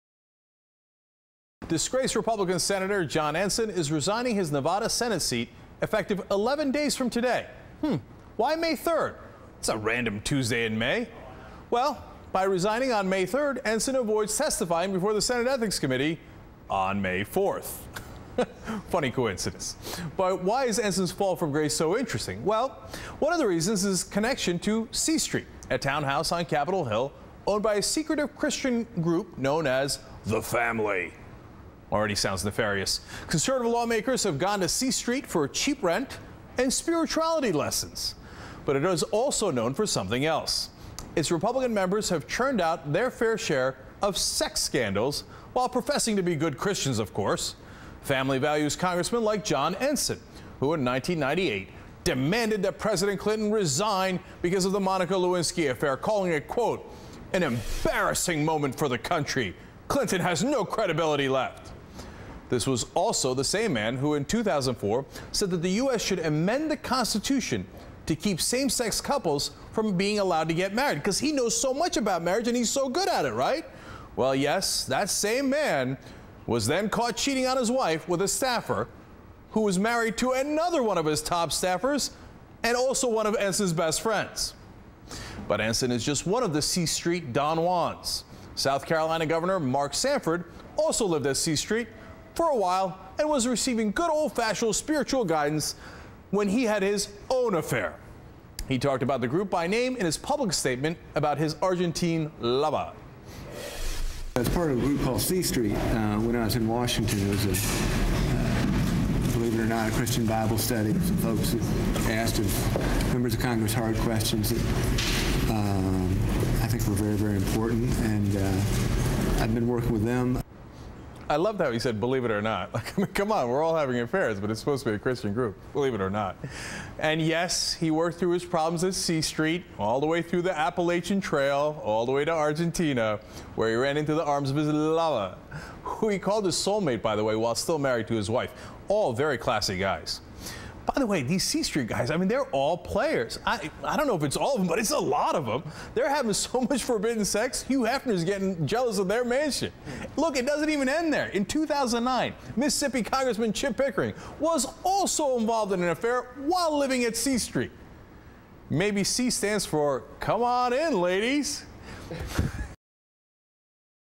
Republican Sexaholics
Cenk Uygur exposes disgraced Republican Senator John Ensign's and Former Governor Mark Sanford's connections to sex scandal prone, christian bible study, The Family owned C Street.